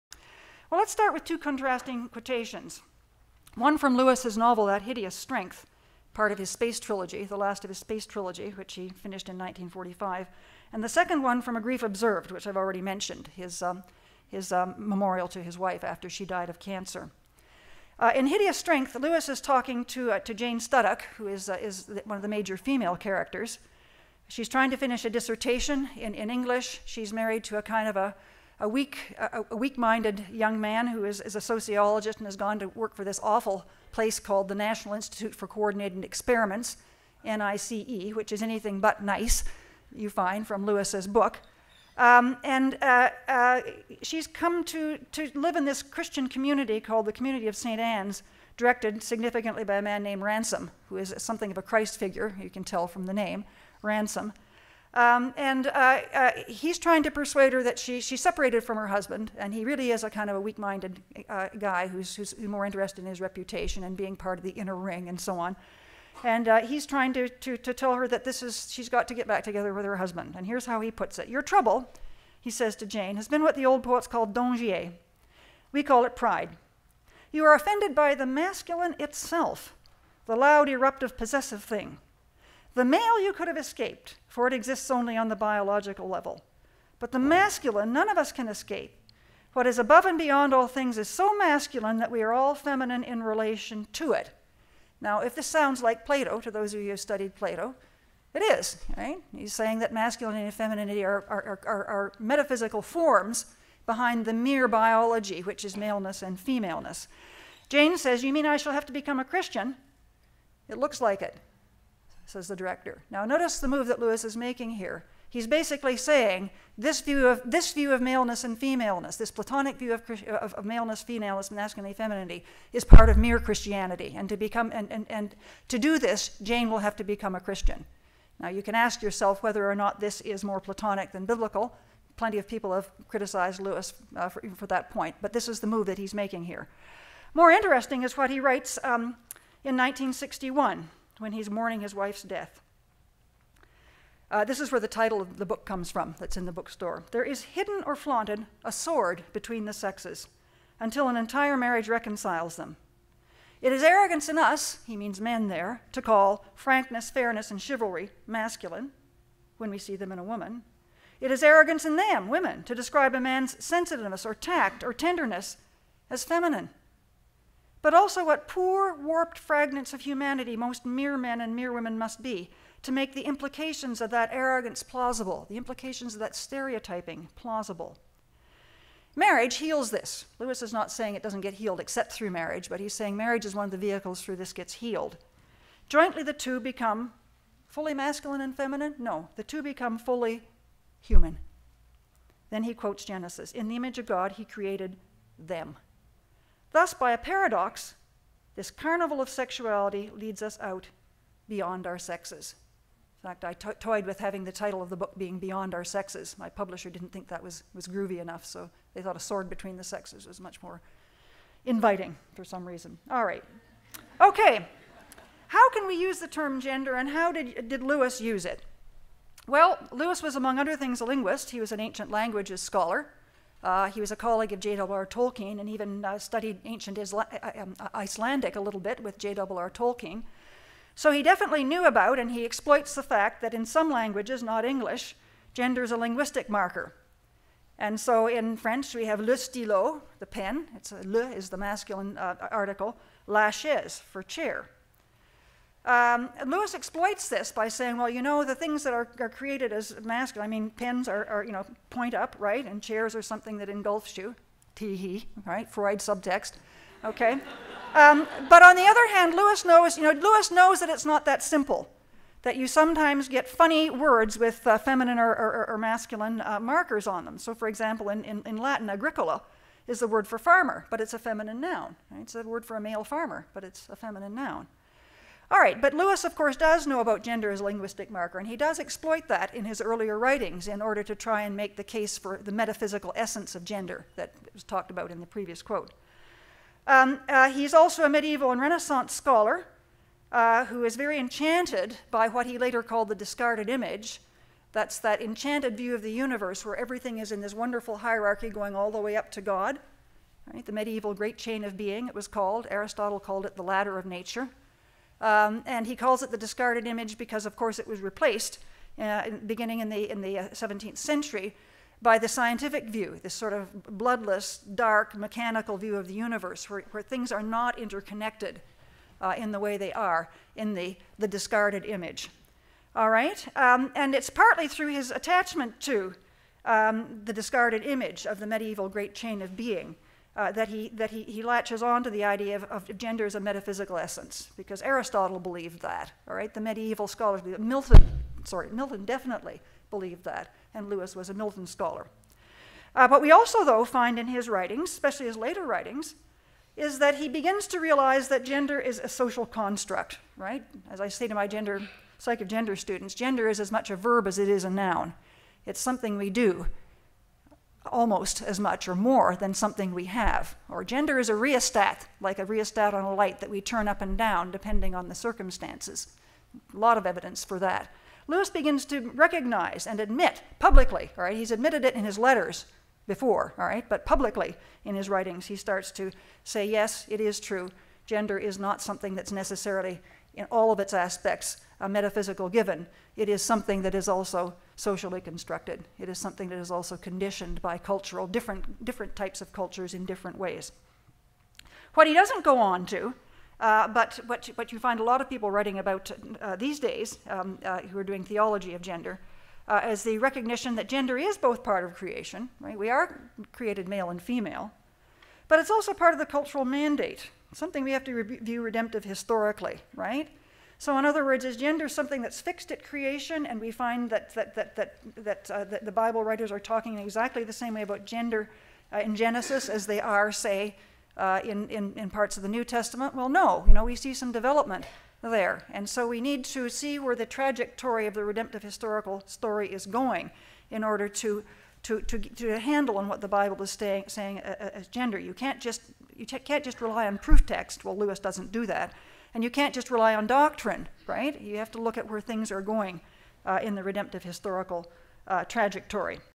Mary Stewart Van Leeuwen - Part 2
October 8: Homecoming Weekend. Mary Stewart Van Leeuwen, EU Professor of Psychology. "A Preference for People: How C.S. Lewis changed his thinking about gender". Mary will discuss her new book A Sword Between the Sexes: C.S. Lewis and the Gender Debates.